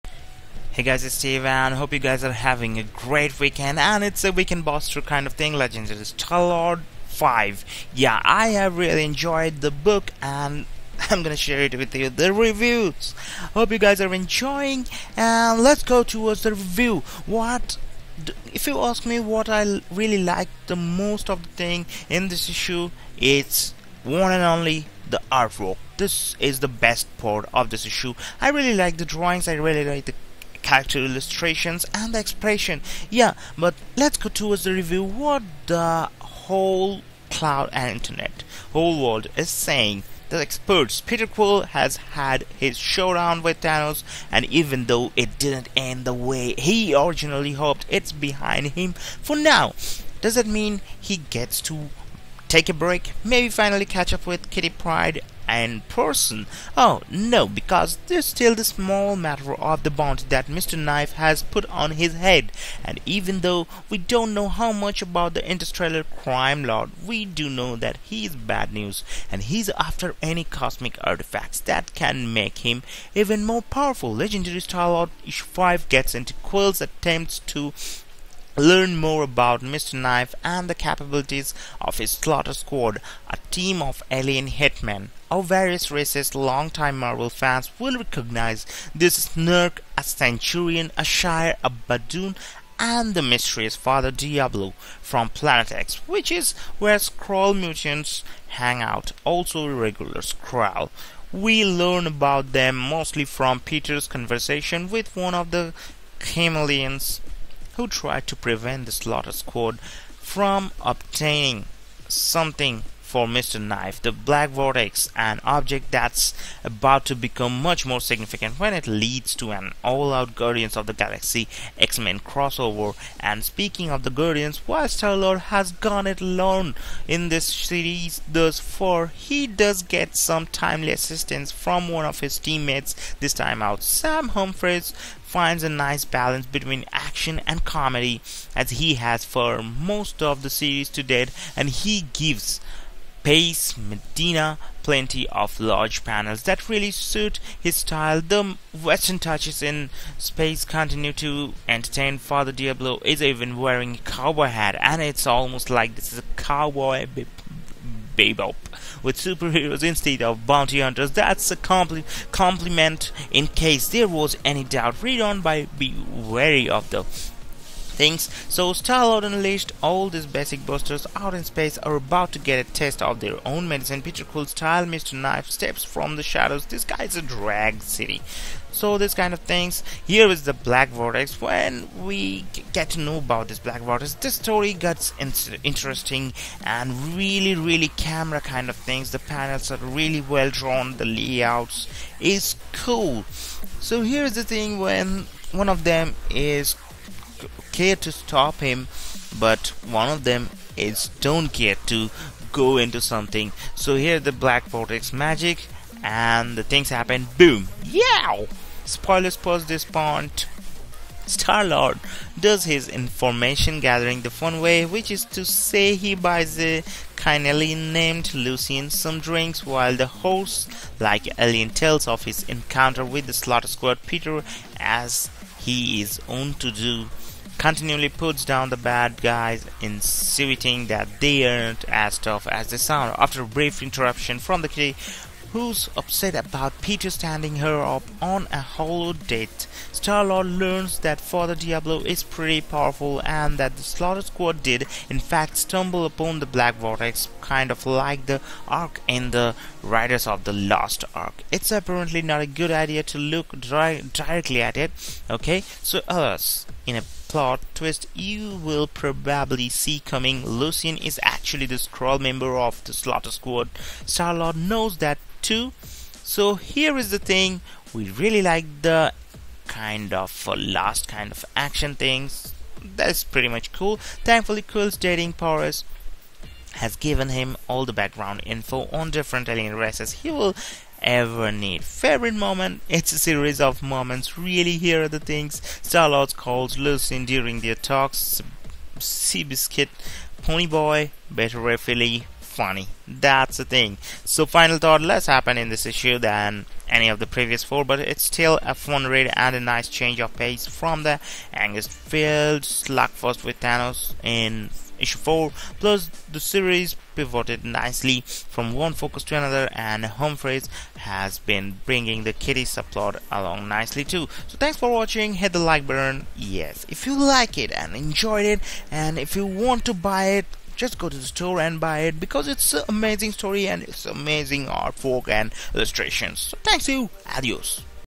Legendary Star-Lord #5 Comic Review
WWWOOOOWwwww!. for the Illustration. COOOLLL
check out the FULL review...and Stay alive with me on

LEGENDARY STAR-LORD (2014) #5
Published: November 05, 2014
Writer: Samuel Ryan Humphries
Cover Artist: Paco Medina
• STAR-LORD UNLEASHED! All these basic busters out in space are about to get a taste of their own medicine, Peter Quill-style! • MISTER KNIFE steps from the shadows...this guy is drag city. • WHAT is the mysterious Black Vortex? (It’s not a band, it’s an ancient cosmic artifact that is gonna cause big trouble.) • Dude, space is awesome, but the ice cream up here suuuuucks.